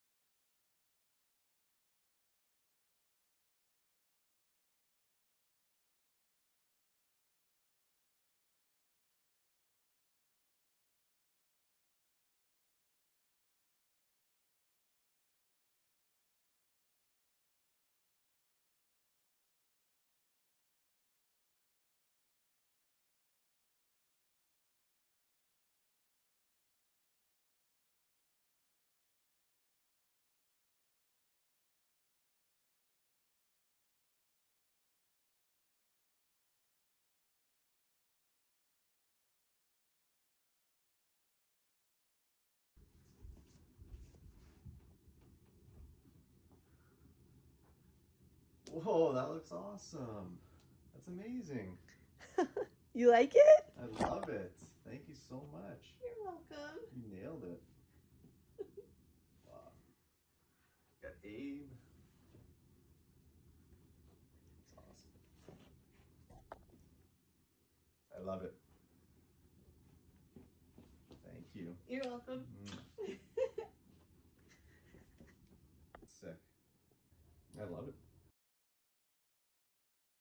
Woman paints flawless Simpsons mural and leaves partner stunned
Trying is the first step toward... crafting a masterpiece, and Rachel Lancaster seems to be well aware of it.

In this spellbinding footage, Rachel uses her wall as a canvas and graces it with her impeccable mural painting chops.

&quot;Worked on a freehand Simpsons mural,&quot; she commented.

The awe-inducing mural depicts Homer Simpson and others having a fun night at a bar.

From doing full justice to the physical traits of the characters to nailing the background, Rachel hits it out of the park with this masterpiece.

Upon completing the mural, she invites her partner, Eric, to take a look at it, and he is completely mesmerized the moment it catches his eye.

Name: Rachel Lancaster
Location: Winnipeg, Canada